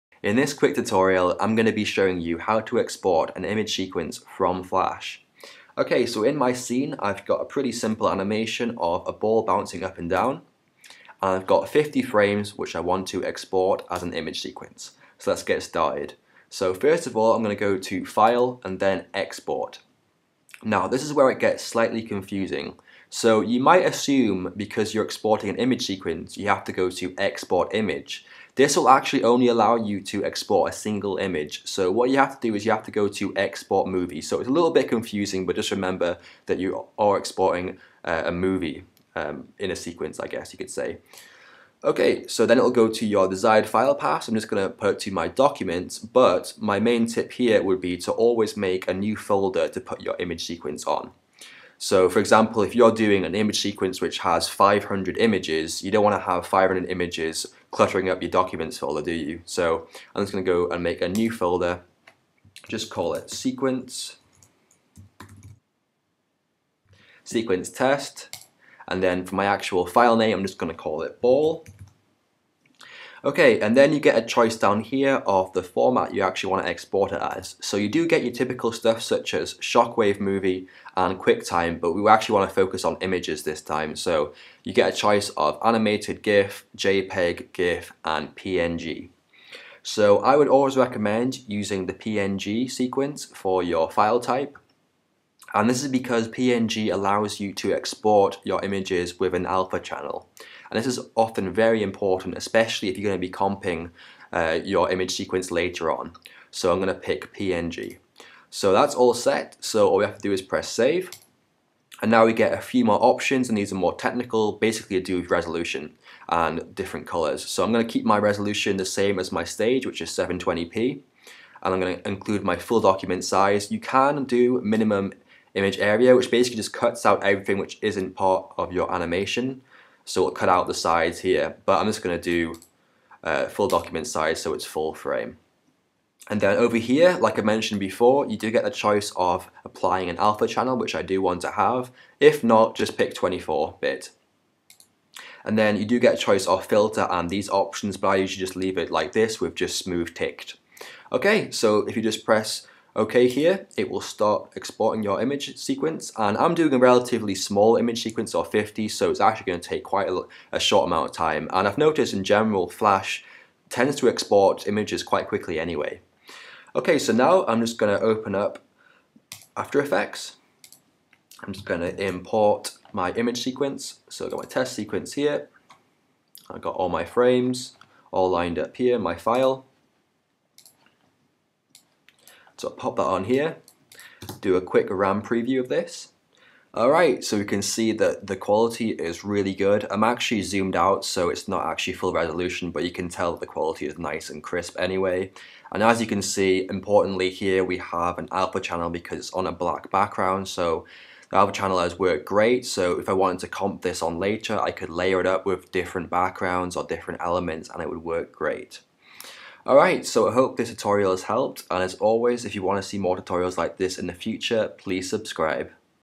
Flash Tutorial: Exporting an Image Sequence
In this quick tutorial I'll be showing you how to export an image sequence from Flash and some handy tips along the way.